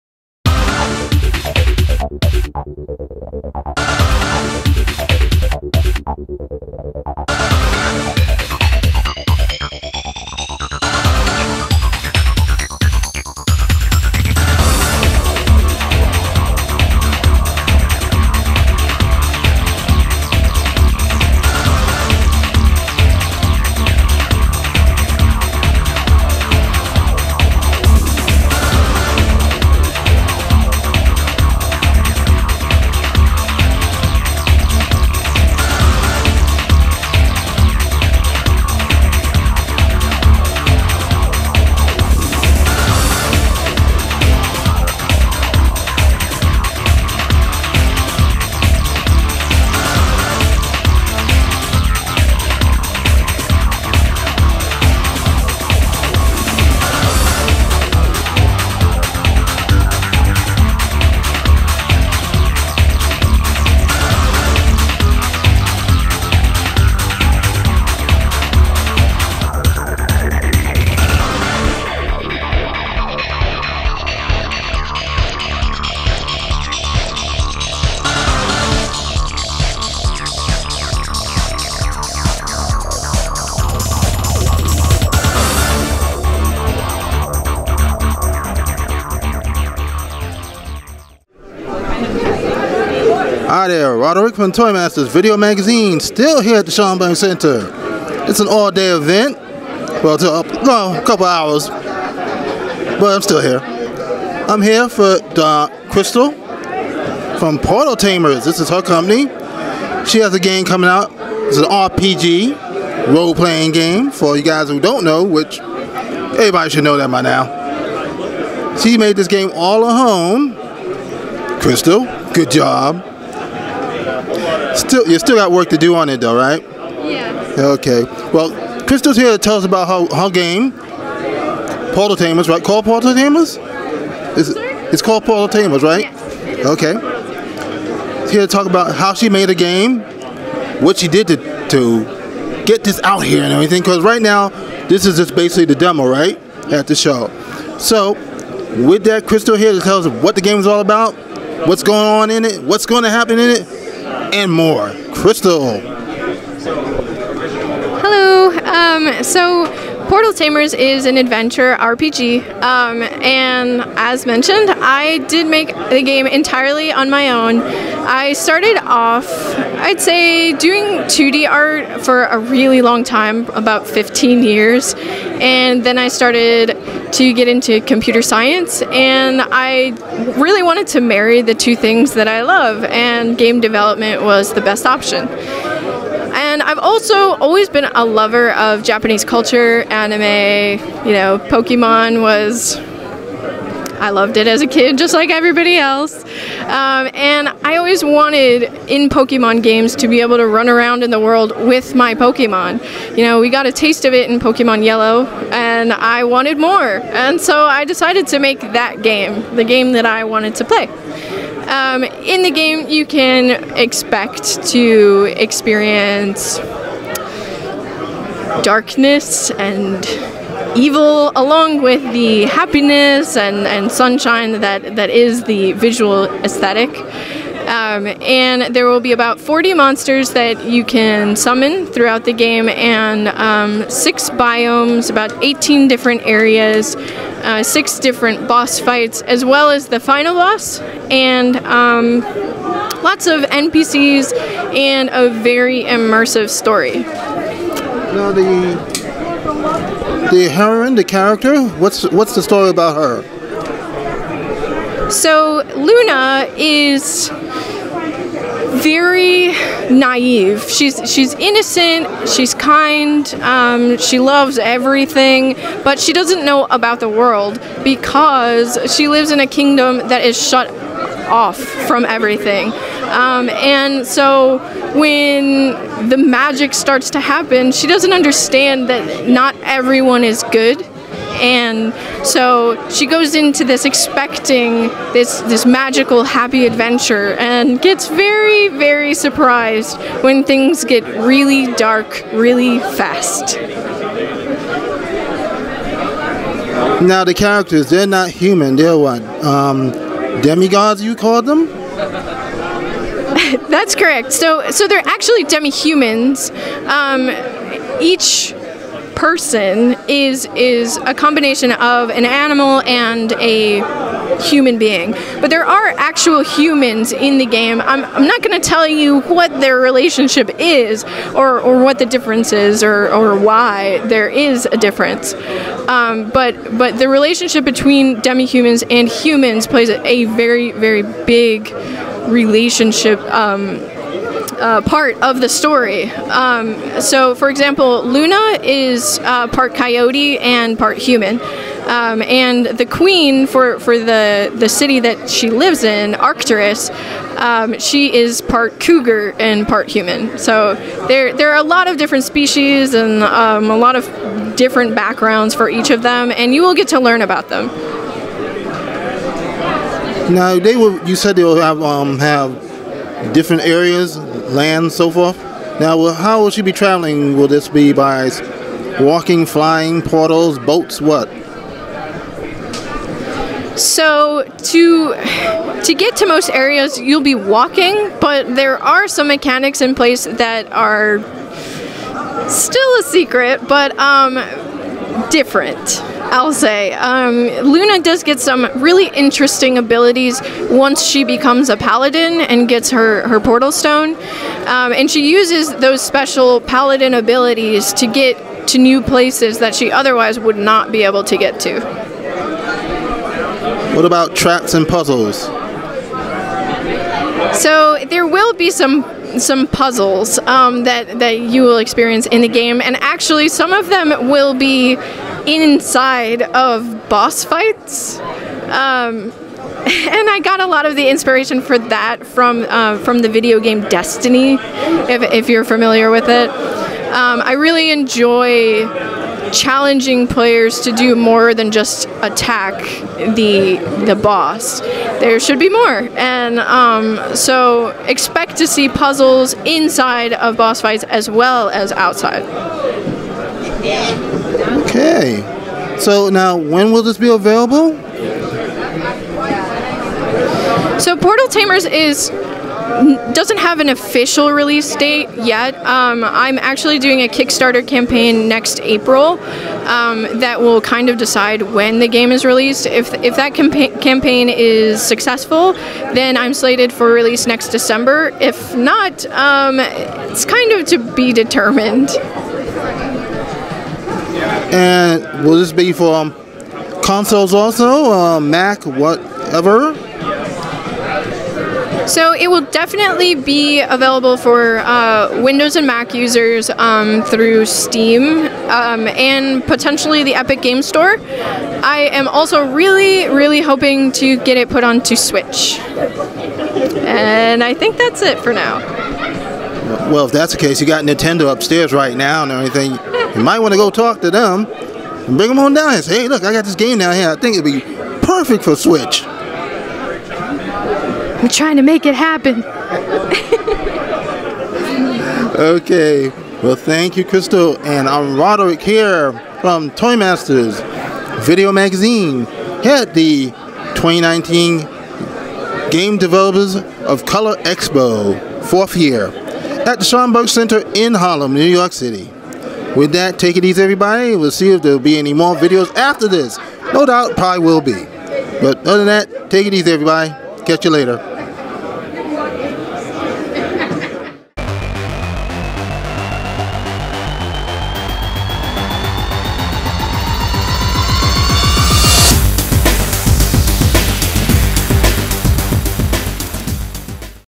Game developer Crystal Sciarrino talks, about her Fantasy RPG Game Portal Tamers at Game Devs of Col
Crystal talk about why she made the game and the character Luna.